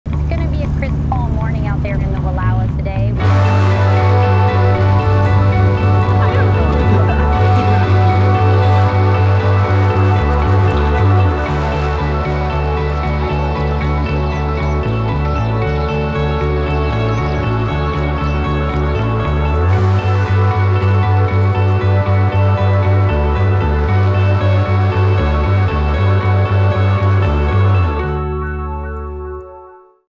Heritage Collection Inspired by Wallowa Mountains | Columbia Sportswear
Inspired by the singular beauty of the Wallowa Mountains of Eastern Oregon, this collection pays homage to the original Wallowa Vest, first introduced in 1978. It features a special edition print, colors highlighting the beauty of the region, and built-in technology to equip your daily adventures. Whether you’re hiking high above a scenic valley or strolling to your favorite coffee spot, the Heritage Collection has you covered.